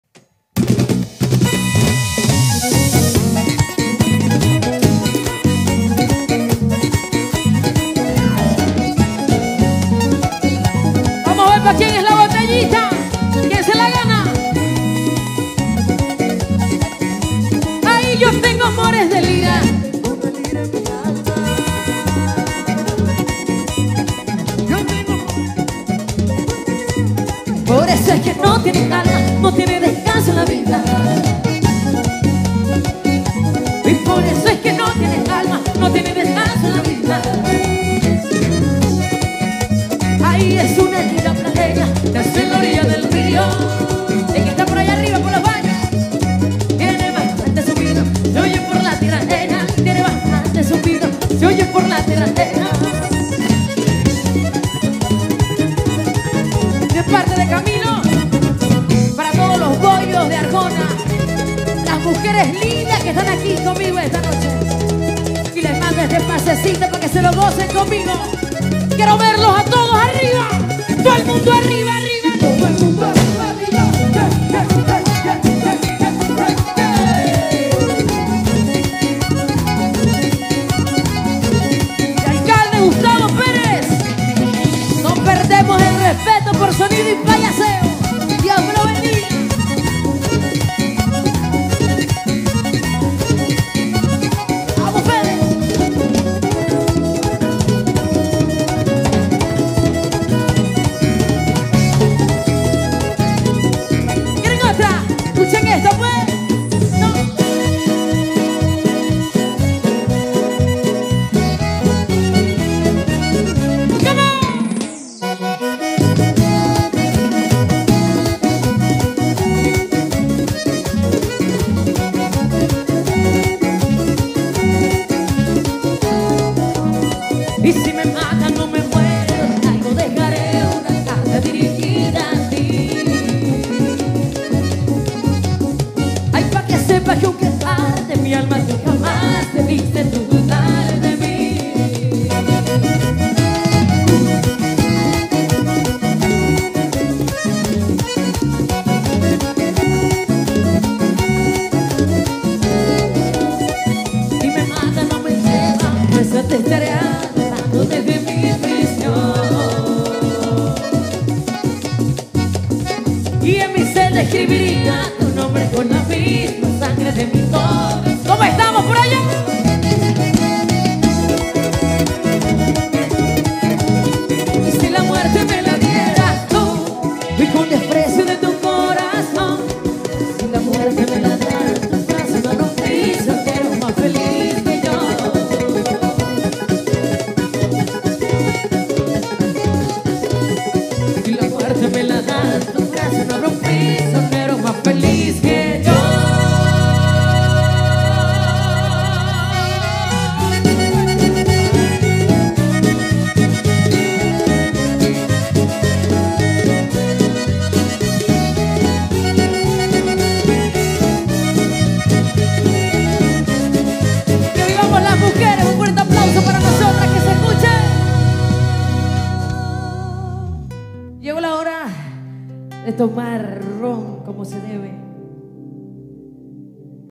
La lira Natalia Curvelo concierto Arjona Bolívar
Lanzamiento de las Fiestas Patronales de Arjona Bolívar 2024